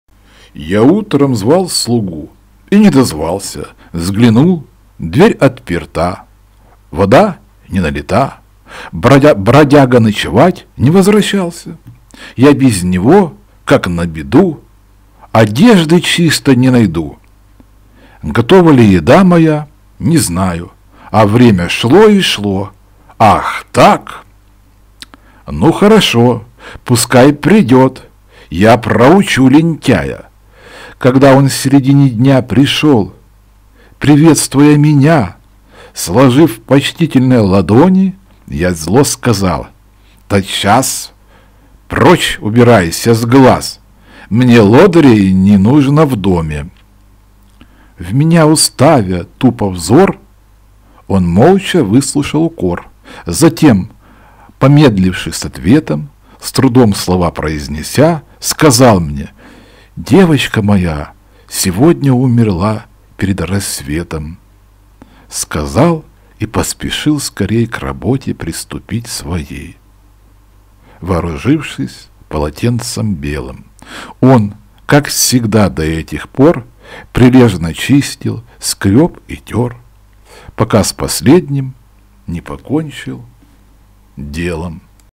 Карма ,Рабиндранат Тагор , Зарубежная Поэзия , читает Павел Беседин
Я утром звал слугу и не дозвался.
Взглянул — дверь отперта. Вода не налита.
Бродяга ночевать не возвращался.
Я без него, как на беду, одежды чистой не найду.
Готова ли еда моя, не знаю.
А время шло и шло… Ах так! Ну хорошо.
Пускай придет — я проучу лентяя.
Когда он в середине дня пришел, привествуя меня,
Сложив почтительно ладони,
Я зло сказал: «Тотчас прочь убирайся с глаз,
Мне лодырей не нужно в доме».
В меня уставя тупо взор, он молча выслушал укор,
Затем, помедливши с ответом,
С трудом слова произнеся, сказал мне: «Девочка моя
Сегодня умерла перед рассветом».
Сказал и поспешил скорей к работе приступить своей.
Вооружившись полотенцем белым,
Он, как всегда до этих пор, прилежно чистил, скреб и тер,
Пока с последним не покончил делом.